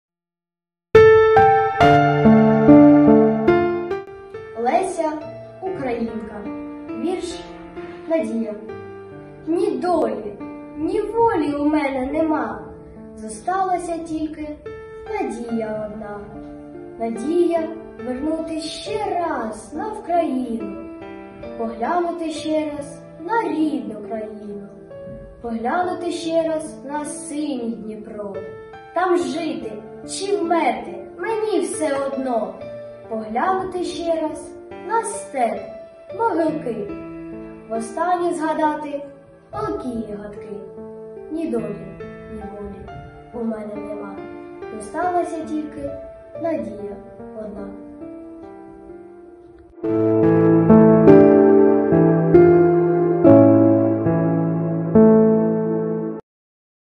Тремба Артем «Надія» | Конкурс «Змагаймось за нове життя!» 2022 рік
Номінація

Тремба Артем
«Надія»

Підготувала - Учитель української мови та літератури вищої категорії Гусарова Любов Василівна.
Дерезуватська гімназія Первомайського ліцею Іларіонівської селищної ради Дніпропетровської області

Заклад - Дерезуватська гімназія Первомайського ліцею Іларіонівської селищної ради Дніпропетровської області

* Родини Бурбак.